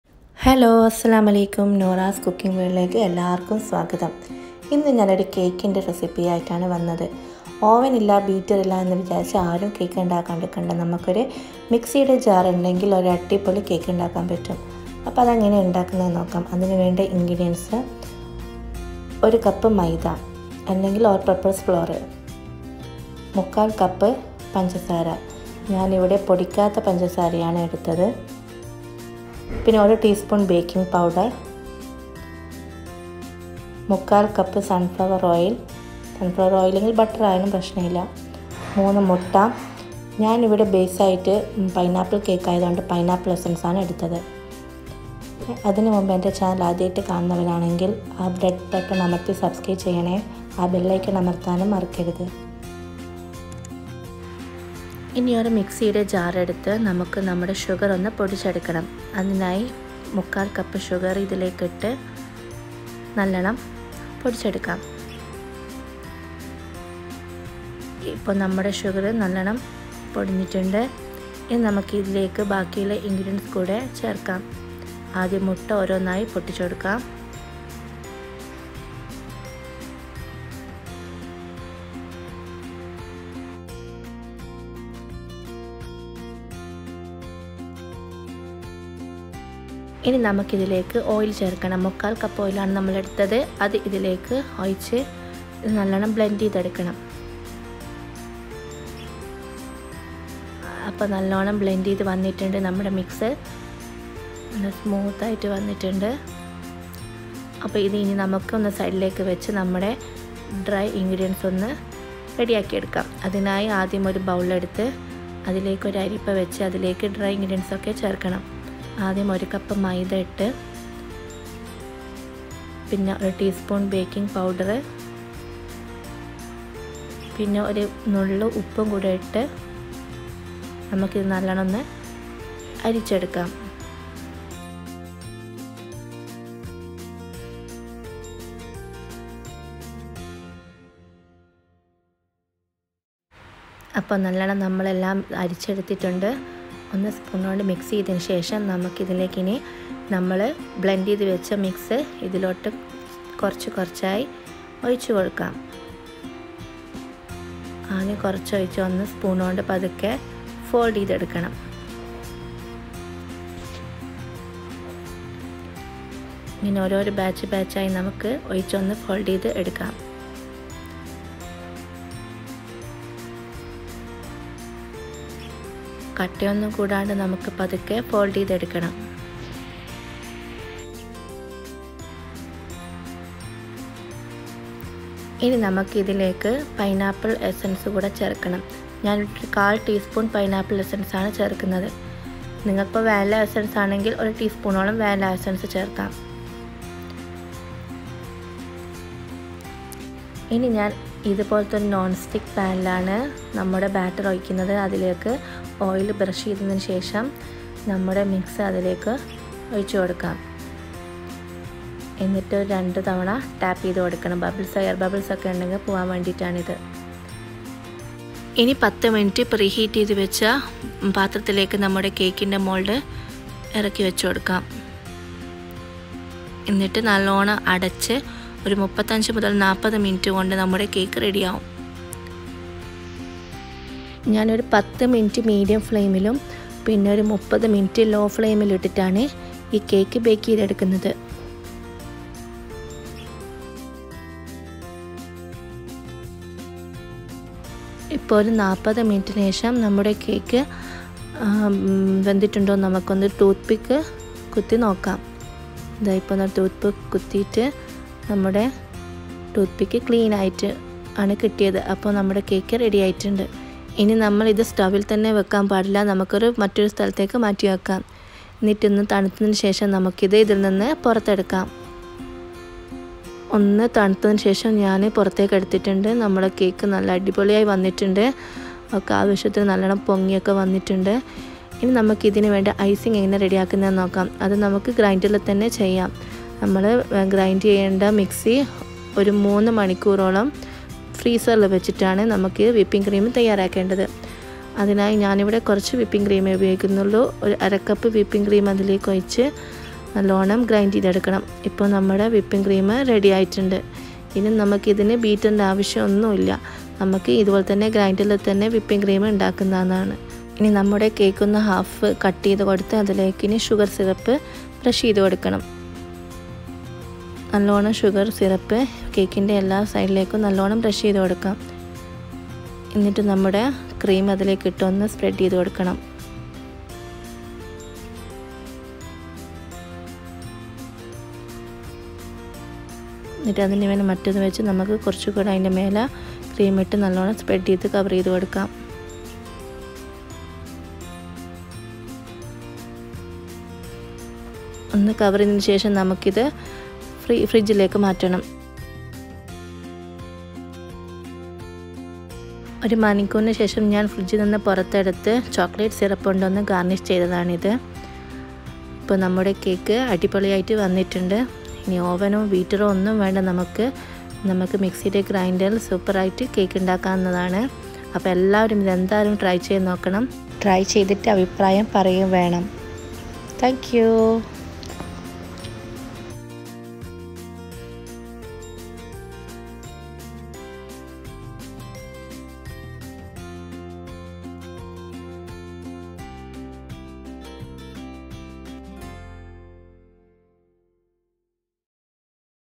No oven, No beater Easy cake in a blender || മിക്സി ഉപയോഗിച്ചിട്ട് ഈസി കേക്ക്‌||nooras cooking world
Without oven and beater Easy cake in a blender. Pls try n enjoy

Ingredients:
Pineapple essence-1/3 tsp
Egg-3

For icing
Whip cream 1/2 cup

Please leave your suggestions and comments in the comment section below ..